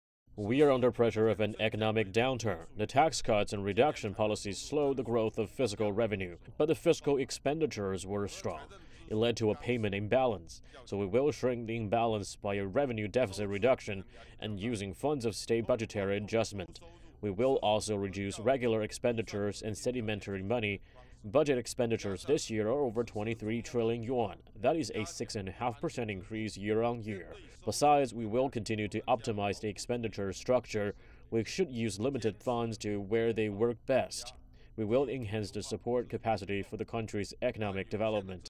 Finance Minister to balance fiscal revenue and expenditure
Finance Minister Liu Kun told the 20th China Development Forum (CDF) that China will take steps to reduce the nation's fiscal imbalance.